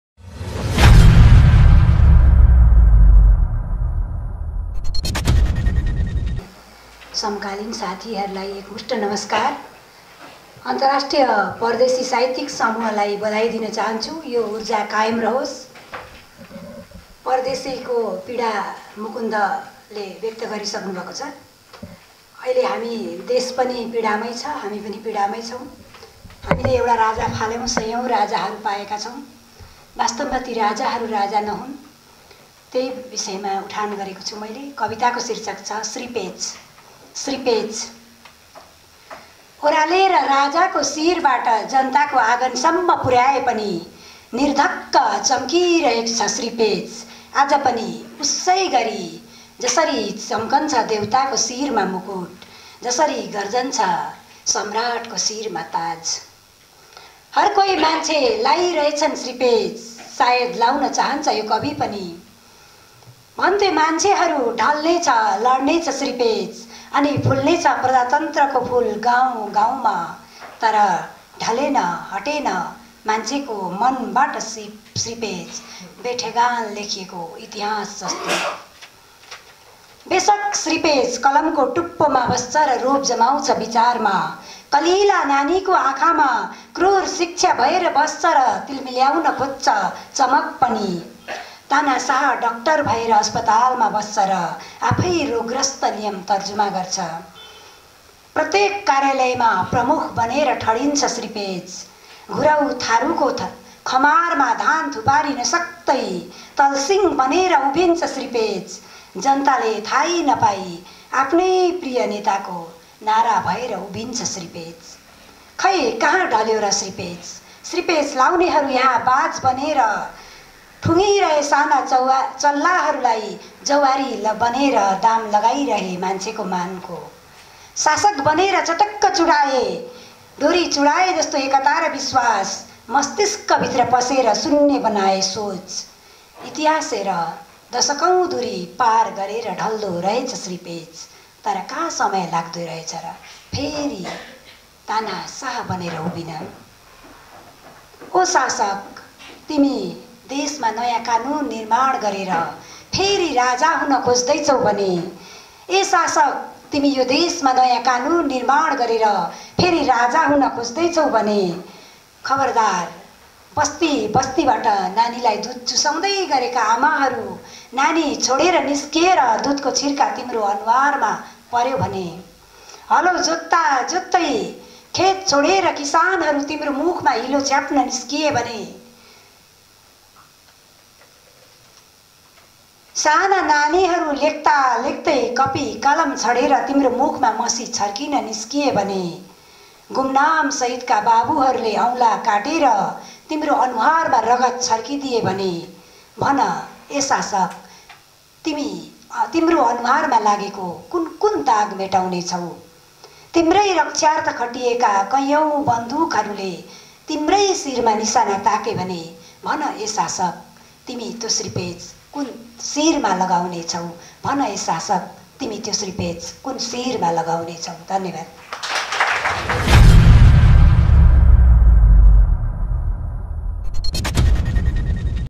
निर्धक्क चम्किरहेकाे छ श्रीपेच अाजपनि...... दांगकी कवियत्री संगिता मगरकाे कविता
पौष सात गते शनिबार उद्योग वाणिज्य संघको सभाहल धरानमा भएकाे अन्तर्राष्ट्रिय परदेशी साहित्यिक समूहको नियमित प्रकाशन परदेशी बिम्बहरू--- साहित्यिक वार्षिक पुस्तकको दशौं अङ्क वार्ता विशेषांक पुस्तकको विमोचन तथा अपसास साहित्य सम्मान एवम् पुरस्कार वितरण समारोहमा दांगकी कवियत्री संगिता मगरले वाचन गरेकाे कविता
श्रीपेच. जस्ताकाे तस्तै